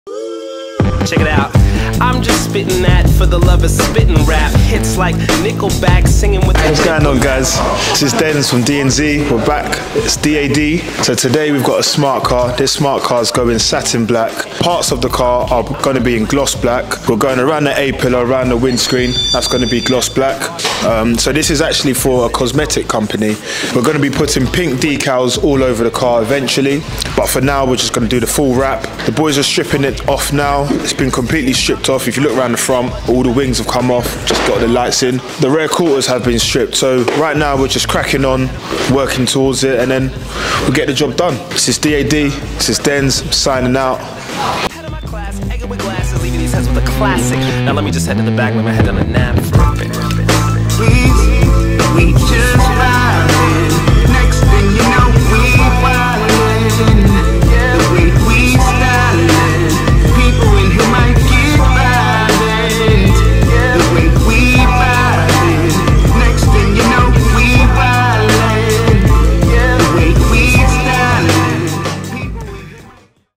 [4K] This Smart Car Is Something Else!!
Today we full-body wrap a Smart Car in Satin Black! A small car takes less time but we pride ourselves on quality. Watch how we transform this stock vehicle into a work of art!